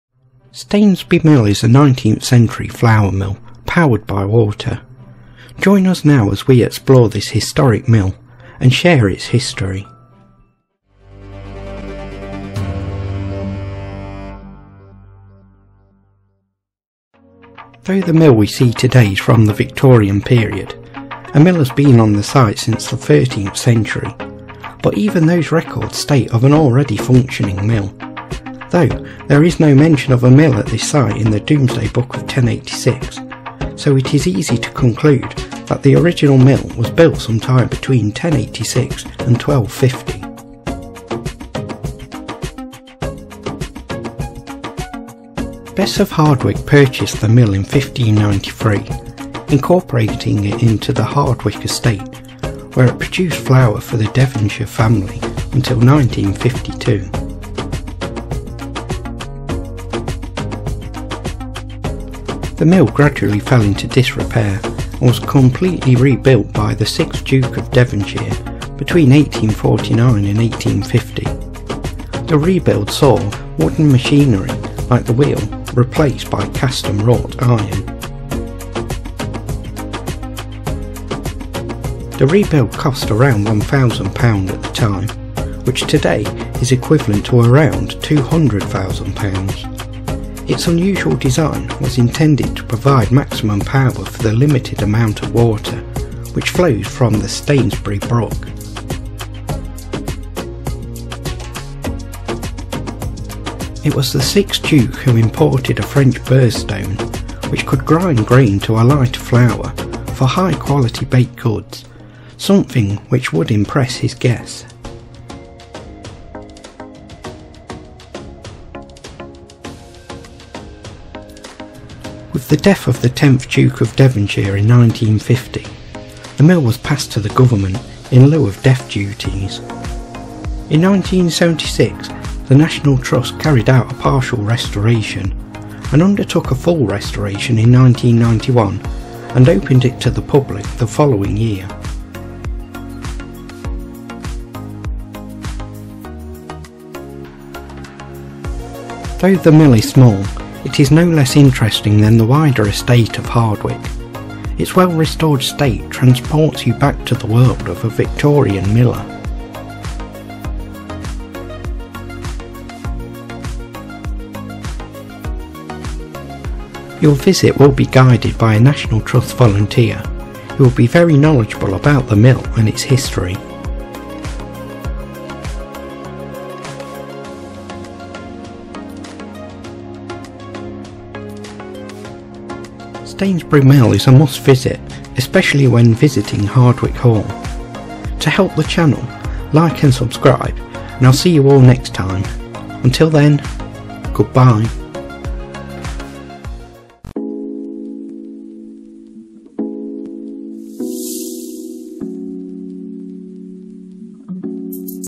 The History Of Stainsby Mill | Hardwick Hall
Stainsby Mill is a 19th-century flour watermill in Doe Lea, Chesterfield, Derbyshire, England. The mill, which has been restored to full working order, is a Grade II listed building and is under the ownership of the National Trust. The mill is part of the Hardwick Hall estate